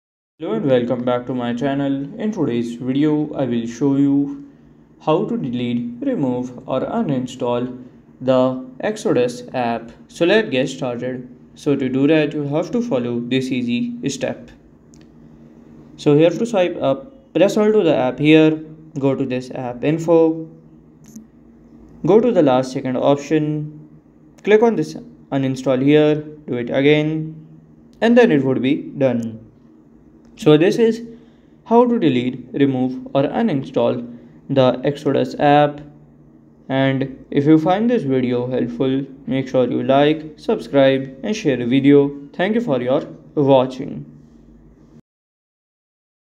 ✅ How To Uninstall/Delete/Remove Exodus: Crypto Bitcoin Wallet App (Full Guide)
In this video, we provide a comprehensive guide on how to uninstall/delete/remove the Exodus: Crypto Bitcoin Wallet app from your device. Follow along to understand the steps and tips for successfully removing the app.

Key Learning Points:
• Steps to uninstall the Exodus app
• Tips for successful app removal